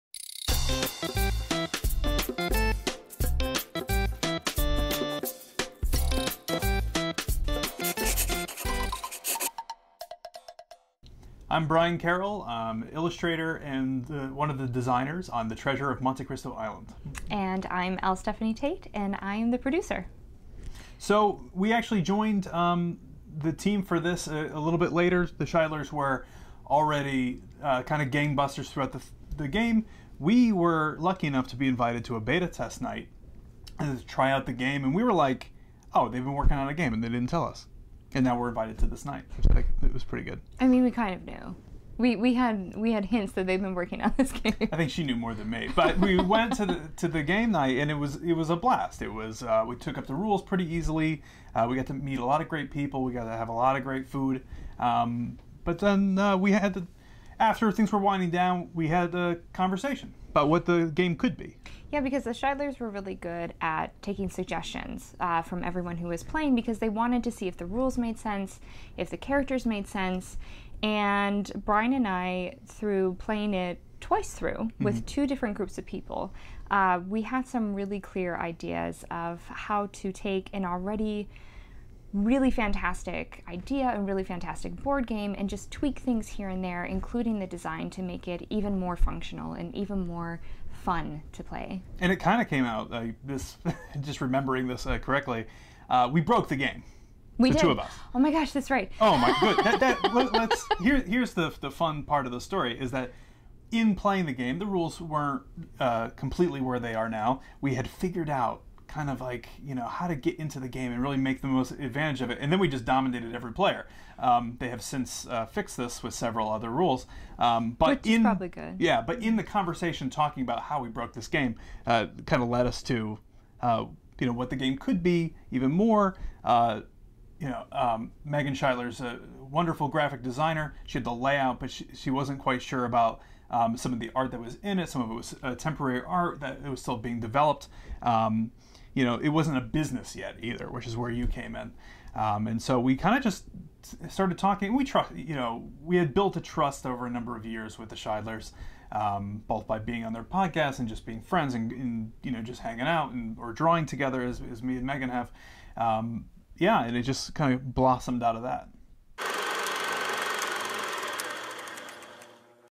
Behind the Scenes Episodes 9 - Meet Brian & Stephanie
After the night of Beta testing, we were presented with the opportunity to bring on two very talented people. Brian Carroll and L. Stephanie Tait came joined us to take the game to the next level.